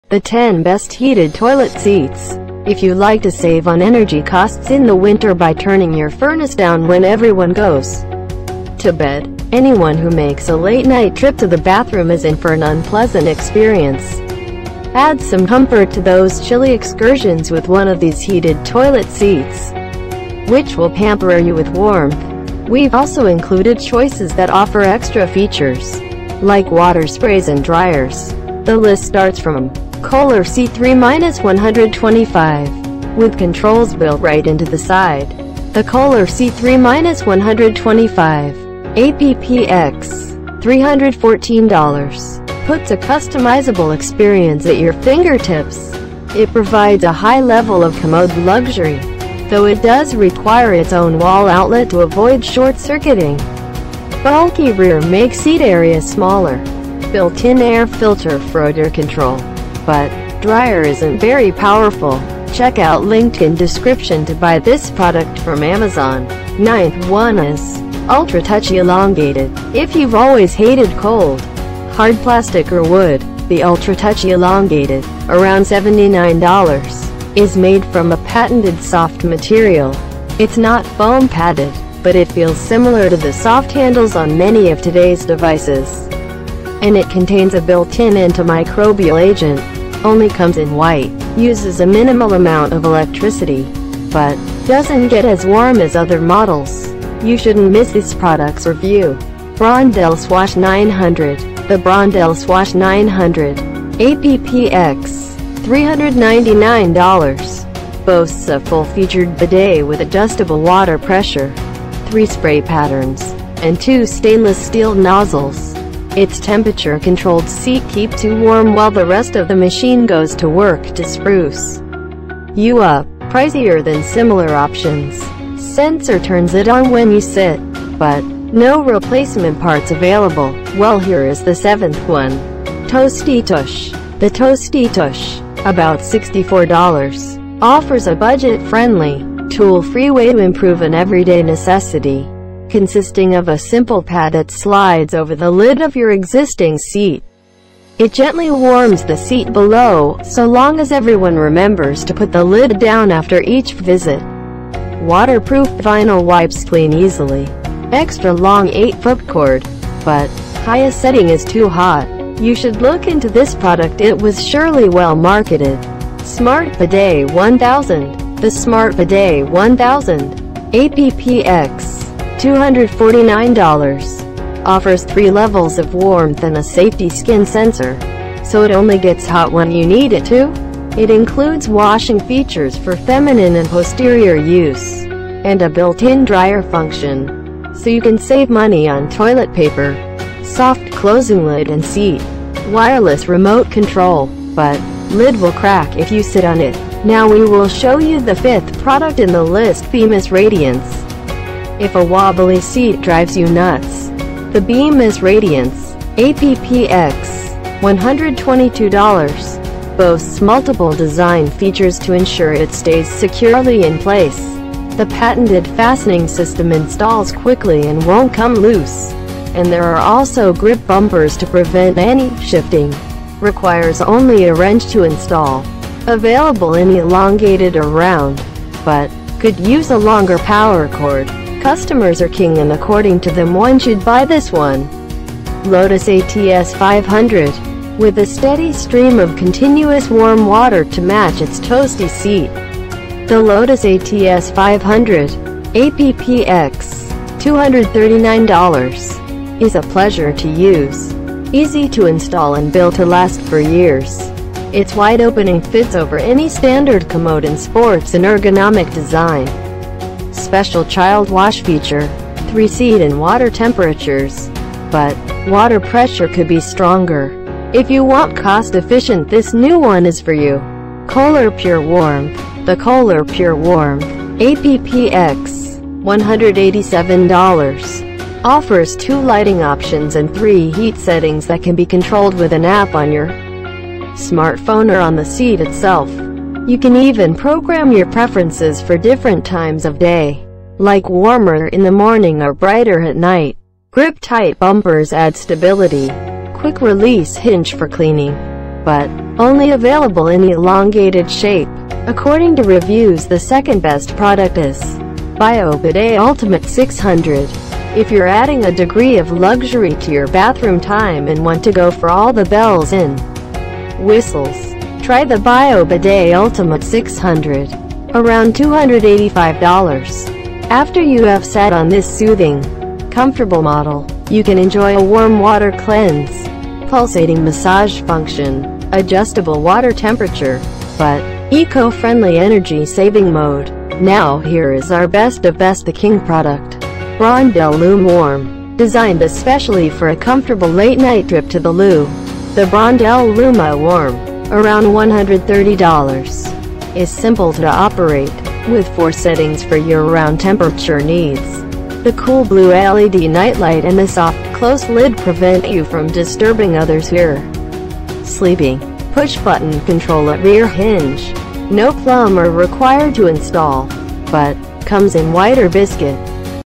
👉 The 10 Best Heated Toilet Seats 2020 (Review Guide)
After 100's of customers and editors reviews of Best Heated Toilet Seats, we have finalised these Best 10 products:

👉 1 Brondell Luma Warm
👉 2 Bio Bidet Ultimate 600
👉 3 Kohler Pure Warmth
👉 4 Lotus ATS-500
👉 5 Bemis Radiance
👉 6 SmartBidet 1000
👉 7 Toastie Tush
👉 8 Brondell Swash 900
👉 9 Ultra Touch Elongated
👉 10 Kohler C3-125